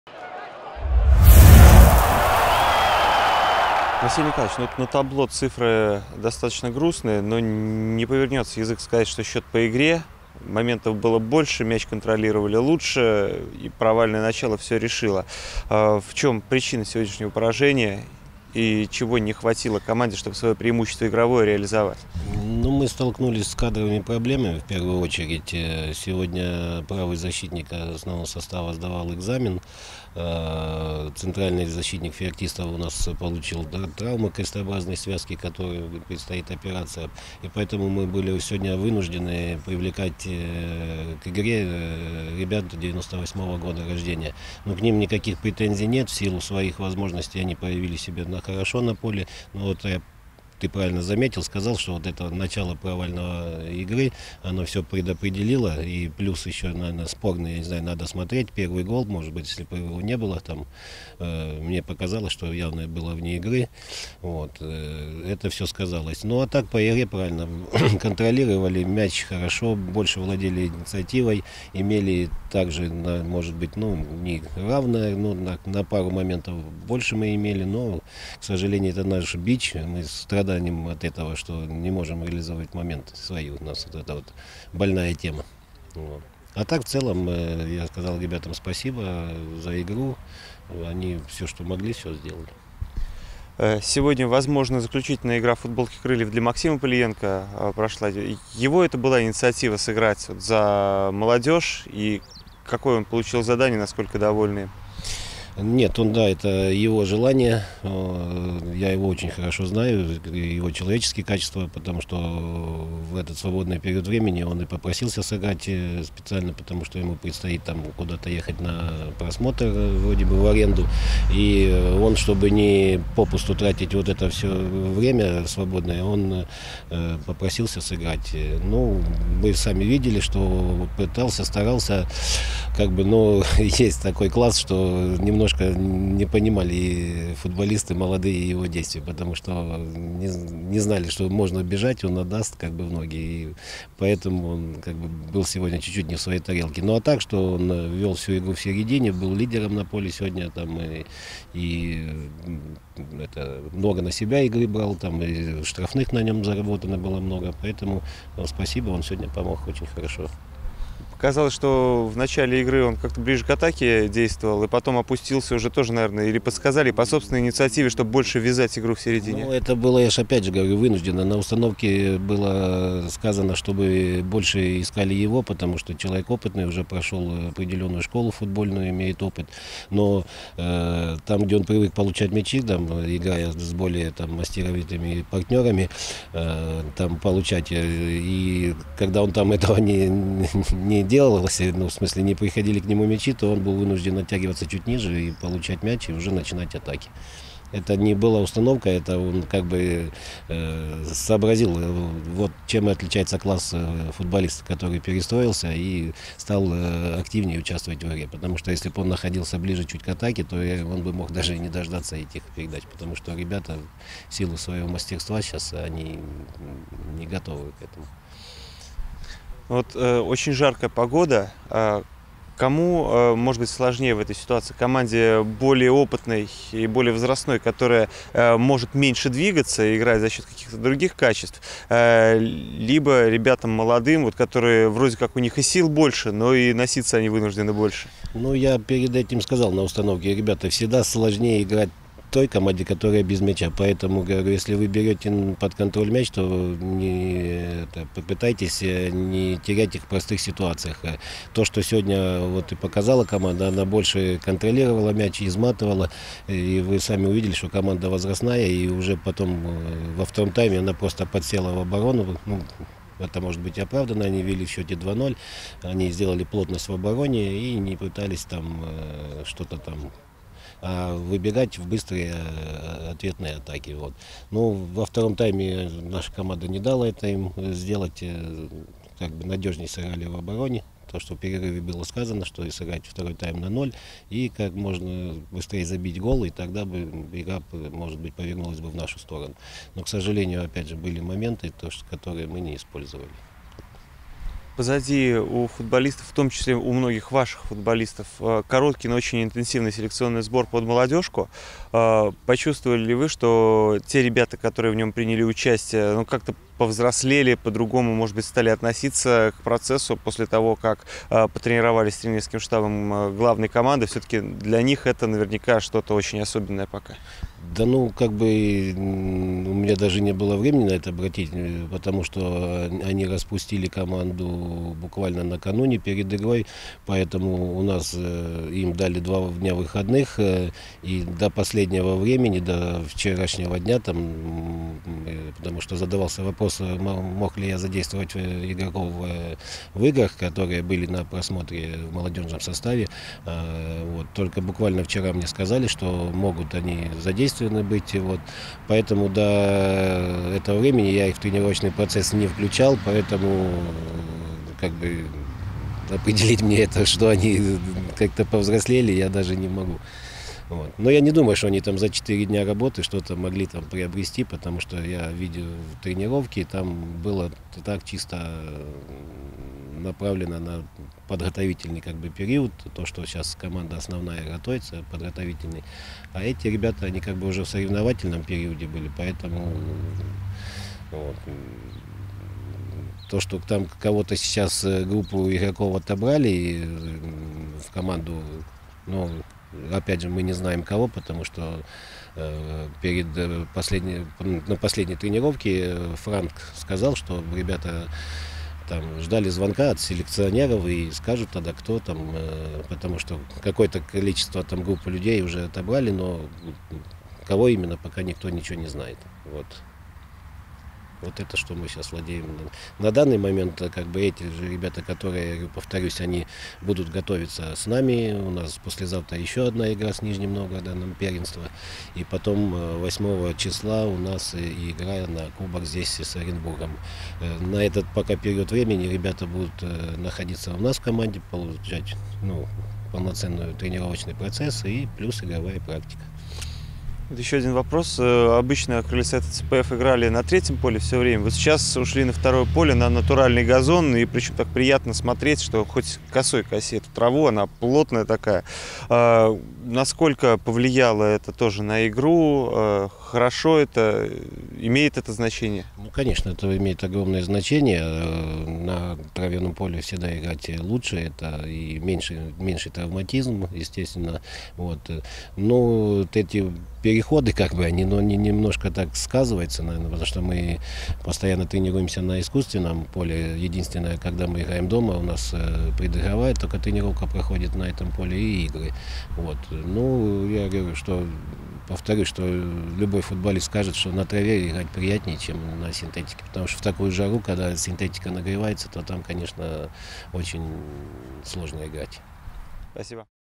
Василий Мазур: Не ожидали, что так так быстро пропустим - "КС-ТВ"
Интервью главного тренера "Крыльев Советов"-ЦПФ Василия Мазура после матча с "Динамо-ДЮСШ".